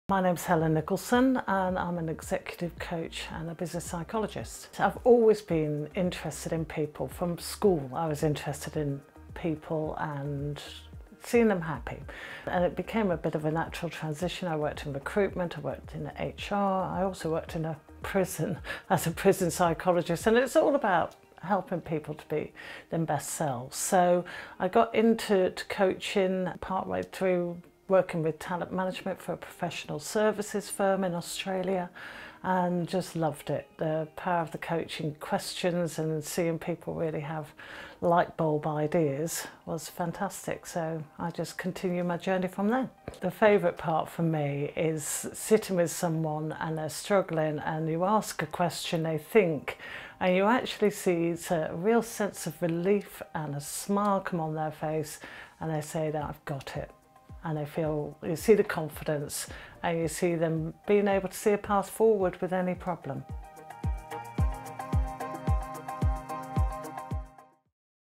Meet Helen Nicholson - technology, arts & entertainment, and manufacturing specialist
Find out more about Helen and what she can offer